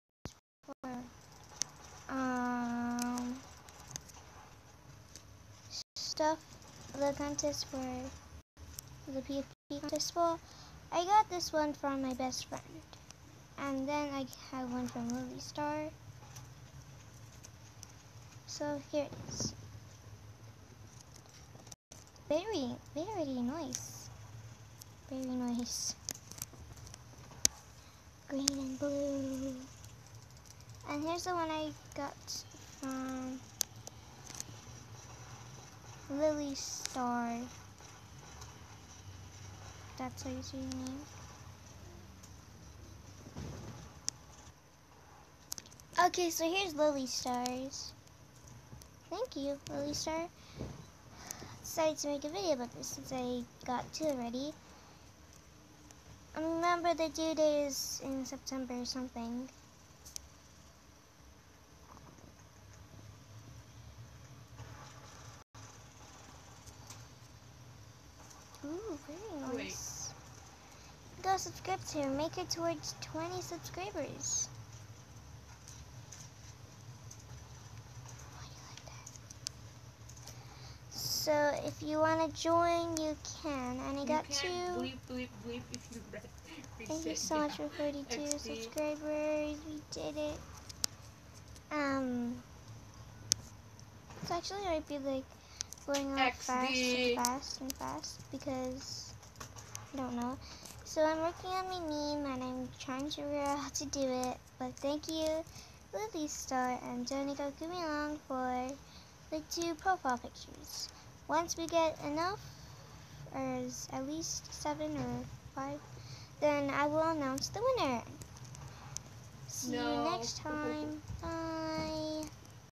I have 2 profile pictures
QwQ, both of them are super good. Its gonna be hard to choose one since u are all amazing artists!